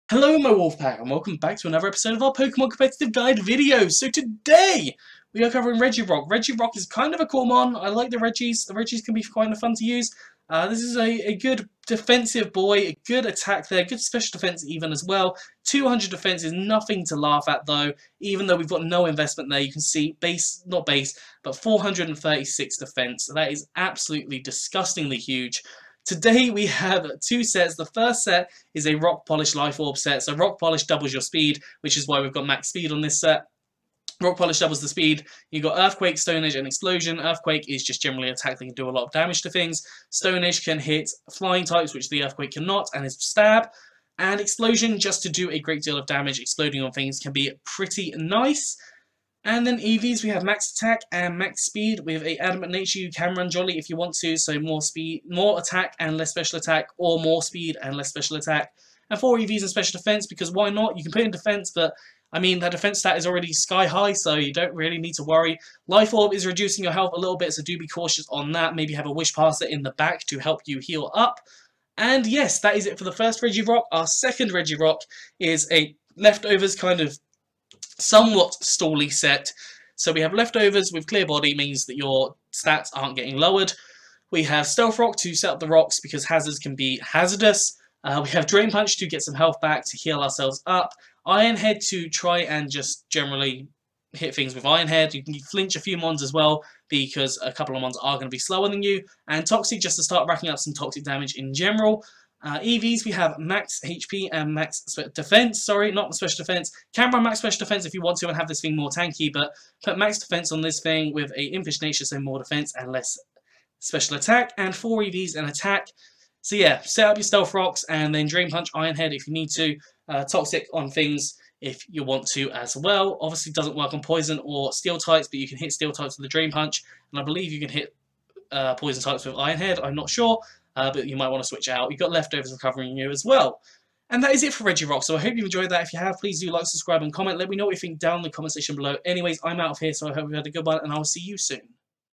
Competitive Guides- How To Use Regirock
Here is a how to series full of competitive sets for all your needs!
There will be some gimmick sets which are here for some light jokes so give them a go if you want!

If you have enjoyed this video and want to show me some support leave a like down below.